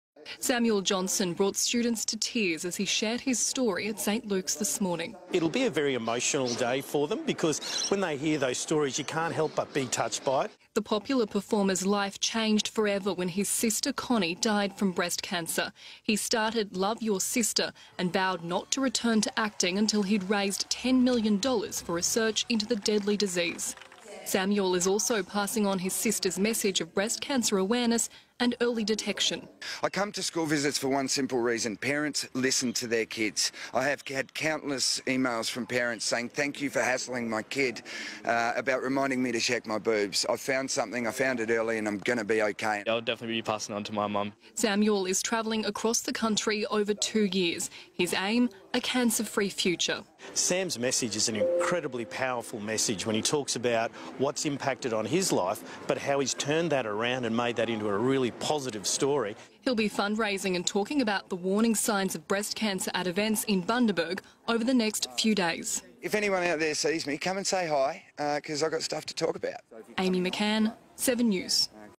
Samuel Johnson has stopped off in Bundaberg on his Love Your Sister tour acro
Samiel Johnson visits St Luke's Anglican School to help spread the LoveYourSister message. A reminder for women to regularly check their lumps for bumps. ( . )( . )